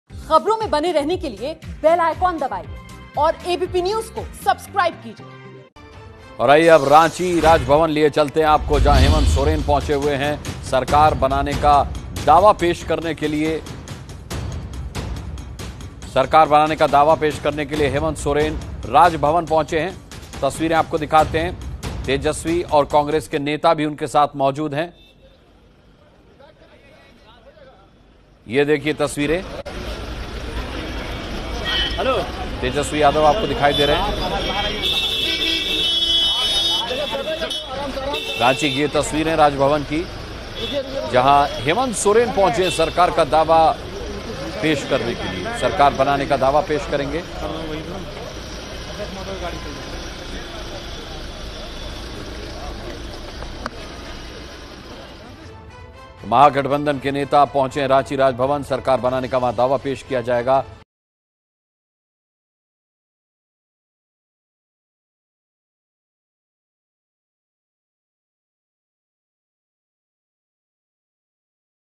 झारखंड: सरकार बनाने का दावा पेश करने राजभवन पहुंचे Hemant Soren
झारखंड में हेमंत सोरेन सरकार बनाने का दावा पेश करने रांची में राजभवन पहुंचे. 3 विधायकों वाली बाबूलाल मरांडी की पार्टी भी समर्थन देगी.